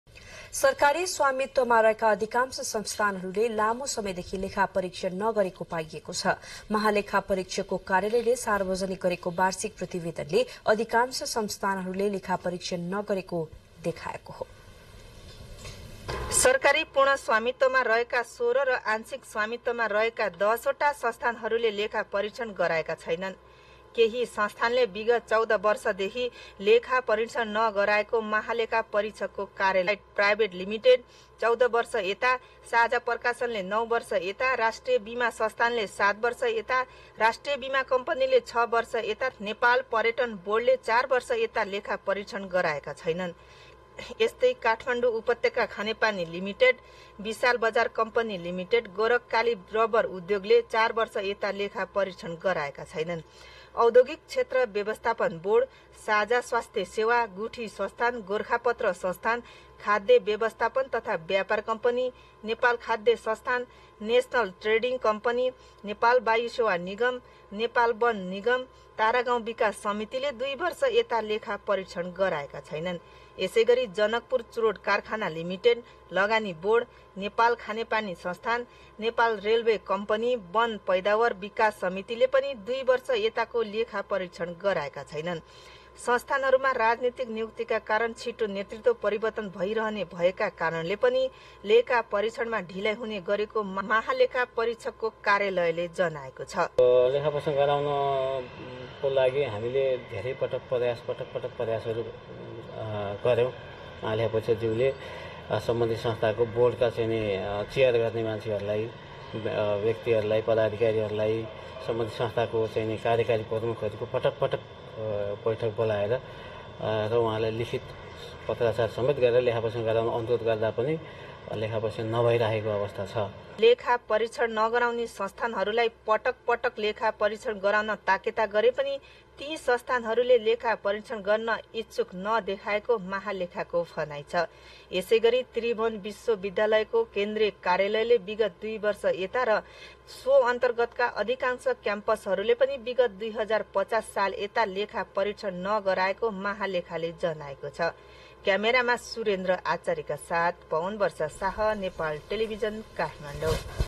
लेखा परिक्षण नगर्ने संस्थानहरु
सरकारी स्वामित्वमा रहेका अधिकांश संस्थानहरुले लामो समयदेखि लेखा परिक्षण नगरेको पाइएको छ । महालेखा परिक्षकको कार्यालयले सार्वजनिक गरेको बार्षिक प्रतिवेदनले अधिकांश संस्थानहरुले लेखा परिक्षण नगरे देखाएको हो ।